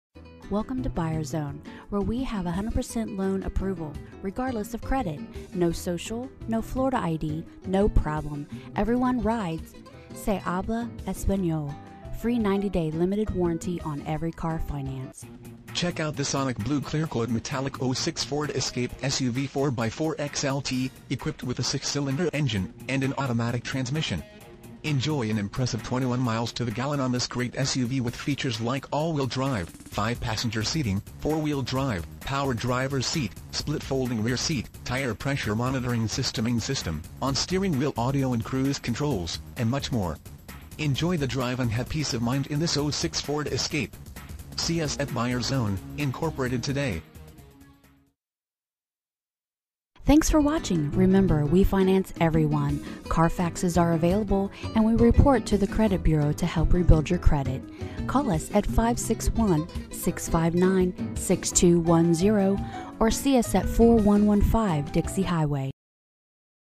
2006 Ford Escape Buyers Zone, Inc. West Palm Beach, FL 33405
Ford Escape Imagine driving this Sonic Blue Clearcoat Metallic Ford Escape SUV 4X4, equipped with a 6 Cylinder engine and an automatic transmission. Enjoy an impressive 21 miles to the gallon on this great SUV with features like All Wheel Drive, 5 Passenger Seating, Privacy Glass, 4 Wheel Drive, Power Driver's Seat, Split Folding Rear Seat, Tire Pressure Monitoring System, on steering wheel audio and cruise controls, and much more. Enjoy the drive and have peace of mind in this Ford Escape. See us at Buyers Zone, Inc. today!VIN:1FMYU93136KA02432

Buyers Zone, Inc.
4115 South Dixie Highway
West Palm Beach, Florida 33405